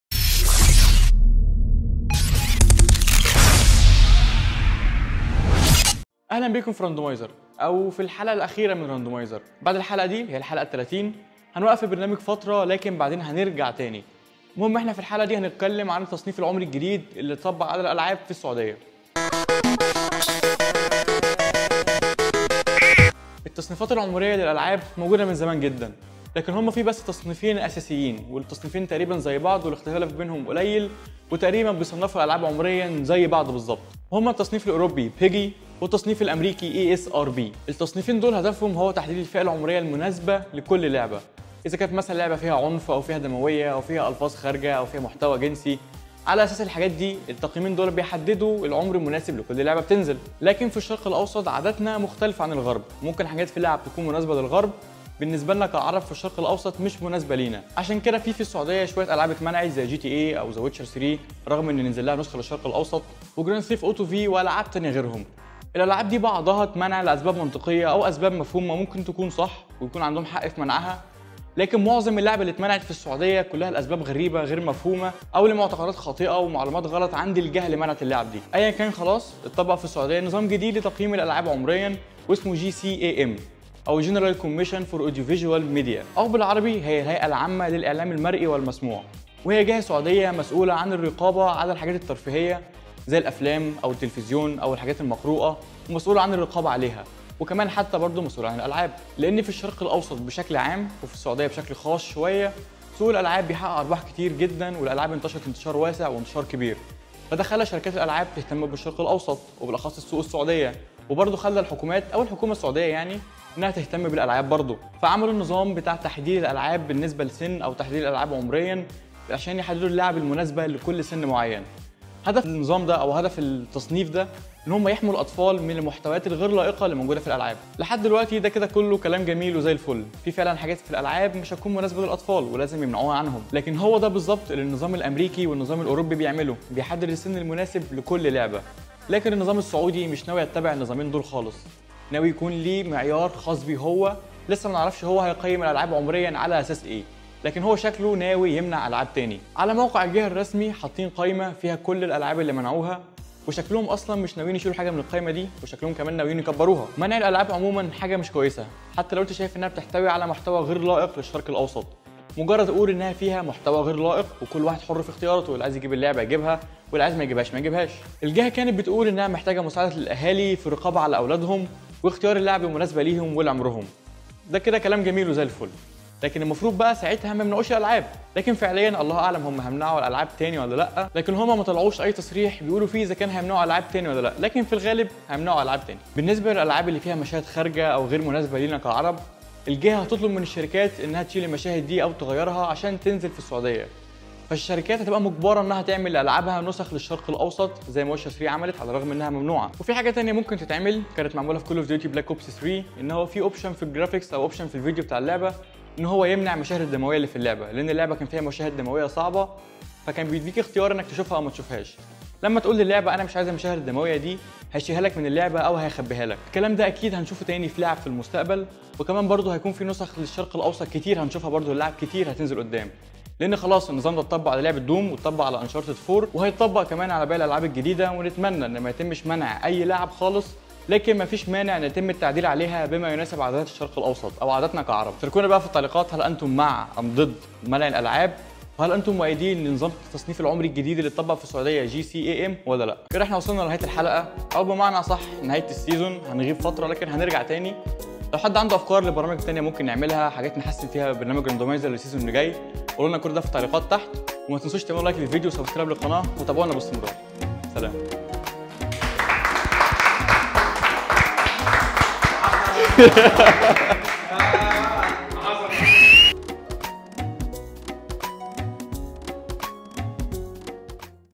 الأخيره Randomizer 30 - منع الالعاب ونظام التقييم العمرى الجديد بالسعودية
تطبيق نظام تصنيف الالعاب عمرياً بالسعودية GCAM وفائدته والهدف منه وهل سيتم منع الالعاب ، سنتحدث عن هذه النقاط فى الحلقة الاخيره من الموسم الاول لراندوميزر، شاركونا رأيكم فى البرنامج واقتراحاتكم لتحسينه